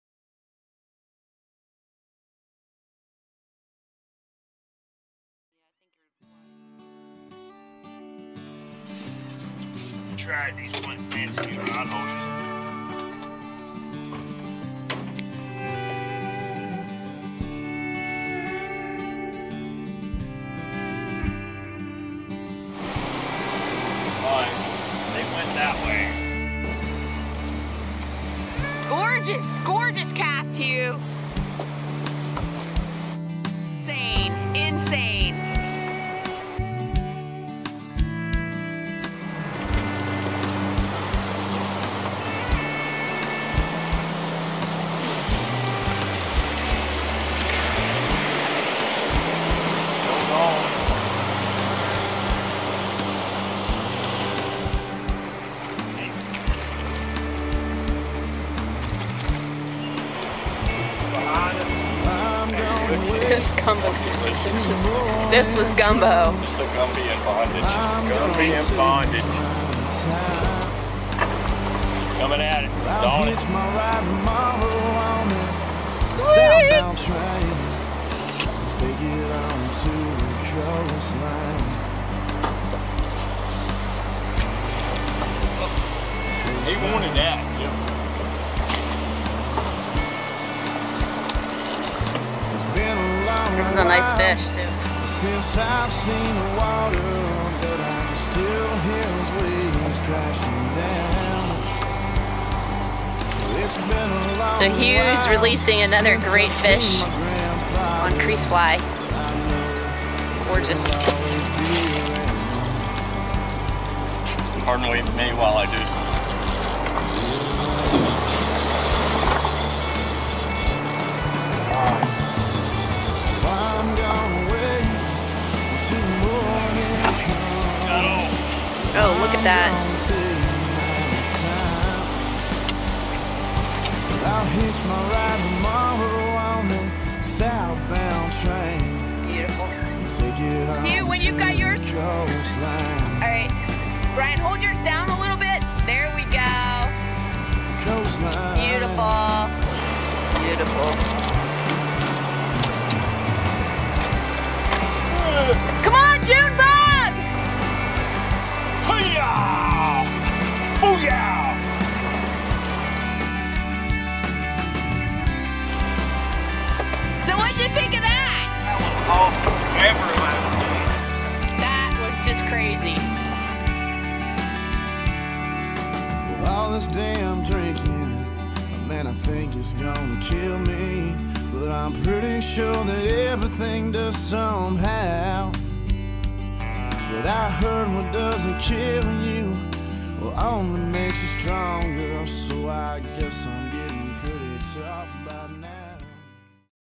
reds in shoals HD
fly fishing for reds in the shoals